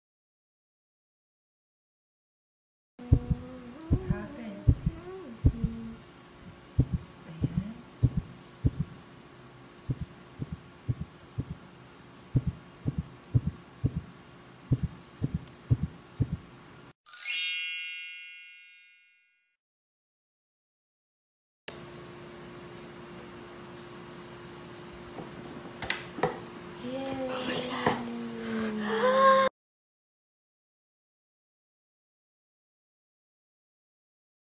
Undoing Changes made to your computer ⁉️| Lenovo- Repair you can do yourself 👍
It was successful and thank God the laptop of my daughter was FIXED 💯

This video was based on our own experience at first we got worried 💭  but then
we patiently waited, that’s all we did 👍😊

Waiting isn’t bad 🙂
“There is a time for everything” - Ecclesiastes 3